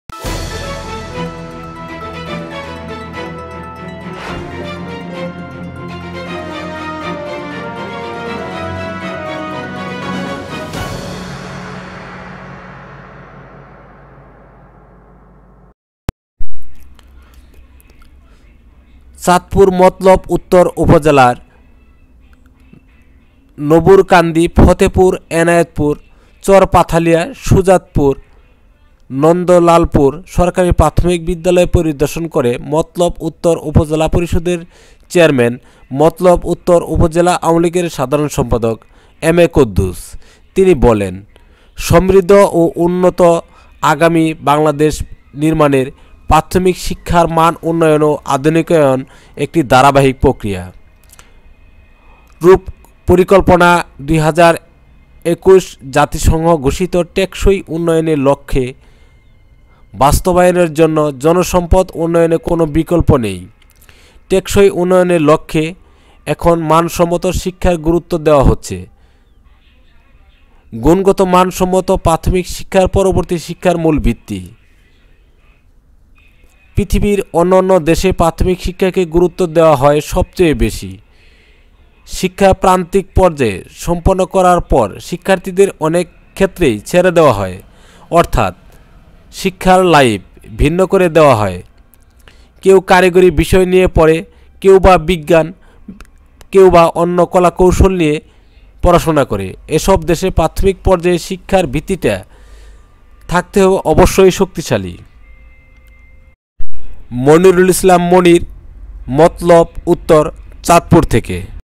মতলব উত্তরের বিভিন্ন শিক্ষাপ্রতিষ্ঠান পরিদর্শনে উপজেলা চেয়ারম্যান বীর মুক্তিযোদ্ধা এমএ কুদ্দুস
মনিরুল ইসলাম মনির মতলব উত্তর, চাঁদপুর

চাঁদপুরের মতলব উত্তর উপজেলার নবুর কান্দি, ফতেপুর, এনায়েতনগর, চরপাথালিয়া, সুজাতপুর, নন্দলালপুর সরকারি প্রাথমিক বিদ্যালয় পরিদর্শন করে ছাত্র/ছাত্রীদের মাঝে স্কুল ড্রেস, টিফিন বক্স’সহ শিক্ষা উপকরণ বিতরণ ও এনায়েতনগর প্রাইমারী স্কুলে একটি বকুল গাছের চারা রোপন, উঠান বৈঠক ও মা সমাবেশে প্রাথমিক শিক্ষার মানোন্নয়নে দিক নির্দেশনা মূলক বক্তব্য রাখেন, প্রাথমিক শিক্ষা খাতে অনবদ্য অবদান রাখায়, চট্টগ্রাম বিভাগের শ্রেষ্ঠ উপজেলা চেয়ারম্যান হিসেবে নির্বাচিত মতলব উত্তর উপজেলা পরিষদ চেয়ারম্যান, মতলব উত্তর উপজেলা আওয়ামী লীগের সাধারণ সম্পাদক, জাতীয় পানি ব্যবস্থাপনা ফেডারেশনের সভাপতি, মেঘনা-ধনাগোদা সেচ প্রকল্পের সভাপতি বীর মুক্তিযোদ্ধা এমএ কুদ্দুস।

এমএ কুদ্দুস বলেছেন, সমৃদ্ধ ও উন্নত আগামীর বাংলাদেশ বিনির্মাণে প্রাথমিক শিক্ষার মানোন্নয়ন ও আধুনিকায়ন একটি ধারাবাহিক প্রক্রিয়া। রূপকল্প ২০২১ বা জাতিসংঘ ঘোষিত টেকসই উন্নয়ন লক্ষ্য বাস্তবায়নের জন্য জনসম্পদ উন্নয়নের কোনো বিকল্প নেই। টেকসই উন্নয়নের লক্ষ্যে এখন মানসম্মত শিক্ষাকে গুরুত্ব দেওয়া হচ্ছে।

গুণগত মানসম্পন্ন প্রাথমিক শিক্ষা পরবর্তী শিক্ষার মূল ভিত্তি। পৃথিবীর অন্যান্য দেশে প্রাথমিক শিক্ষাকে গুরুত্ব দেওয়া হয় সবচেয়ে বেশি। শিক্ষার প্রারম্ভিক পর্যায়টা সম্পন্ন করার পর শিক্ষার্থীদের অনেক ক্ষেত্রেই ছেড়ে দেওয়া হয়। অর্থাৎ শিক্ষার লাইন ভিন্ন করে দেওয়া হয়। কেউ কারিগরি বিষয় পড়ে, কেউ শিখে বিজ্ঞান বা কলা। এসব দেশে প্রাথমিক পর্যায়ের শিক্ষার ভিতটা থাকে শক্তিশালী, মান থাকে উন্নত।

তিনি আরো বলেন, সময়ের সঙ্গে পরিবর্তিত হচ্ছে আমাদের জীবনধারা। একটা সময় ছিল, বাবা-মায়ের কাছে শিক্ষার প্রাথমিক পাঠ শেষে বিদ্যালয়ে পাঠানো হতো শিশুদের। এখন বাবার সঙ্গে মায়েরও ব্যস্ততা বেড়েছে। চাকরি, সামাজিক দায়িত্ব, সংসার গোছানোসহ নানা কিছুতেই ব্যস্ত হয়ে পড়ছেন আমাদের মায়েরা। ইচ্ছে থাকা সত্ত্বেও অনেক মা তাদের সন্তানকে প্রাথমিক পাঠটুকুুও দিতে ব্যর্থ হচ্ছেন। এসব ক্ষেত্রে প্রাক-প্রাথমিক শিক্ষাকেন্দ্র গুরুত্বপূর্ণ ভূমিকা রাখছে। এই বিষয়টি মাথায় রেখেই সরকার প্রাথমিক বিদ্যালয়ের সঙ্গে প্রাক-প্রাথমিক শিক্ষাকেন্দ্রও চালু করেছে। এটি প্রাথমিক শিক্ষার মানোন্নয়নে বর্তমান সরকারের একটি যুগান্তকারী পদক্ষেপ।

এ সময় উপস্থিত ছিলেন, উপজেলা নির্বাহী কর্মকর্তা এএম জহিরুল হায়াত, উপজেলা শিক্ষা অফিসার ইকবাল হোসেন ভূঁইয়া, সহকারী শিক্ষা অফিসার মাহফুজ মিয়া, সহকারী শিক্ষা অফিসার মোহাম্মদ অলিউল্লাহ, সহকারী শিক্ষা অফিসার আসাদুজ্জামান, উপজেলা শ্রমিক লীগ সভাপতি মুজাহিদুল ইসলাম সেলিম, সাবেক ছাত্রনেতা মিরাজ খালিদ, উপজেলা ছাত্রলীগ নেতা সুজন ভূঁইয়া, ছাত্রলীগ নেতা মোহাম্মদ নুরুজ্জামান, ফতেপুর পূর্ব আওয়ামী লীগ নেতা জয়নাল আবেদীন, উপজেলা স্বেচ্ছাসেবক লীগ সদস্য সচিব শাহজালাল মাষ্টার, ইসলামাবাদ ইউনিয়ন যুবলীগের সভাপতি মিজানুর রহমান, ইসলামাবাদ যুবলীগ নেতা আকতার হোসেন, ফতেপুর পূর্ব ছাত্রলীগ নেতা কামরুল হাসান, ফতেপুর পূর্ব যুবলীগ নেতা দেওয়ান ইসমাইল, দেওয়ান দেলোয়ার হোসেন, আনাস প্রধান, ছাত্রলীগ নেতা রাজিব, ফতেপুর পূর্ব ছাত্রলীগ নেতা জনি’সহ অন্যান্য নেতৃববৃন্দ।